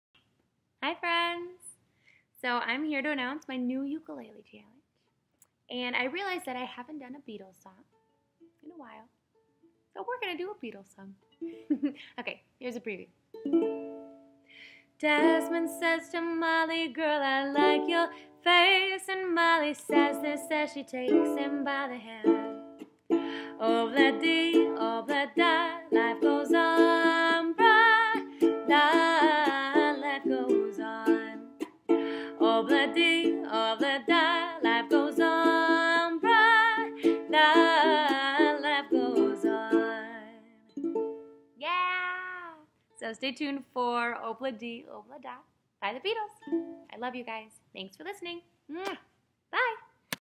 Announcing my New Ukulele Challenge!
Check out what my newest ukulele challenge will be... it's BEATLES! :D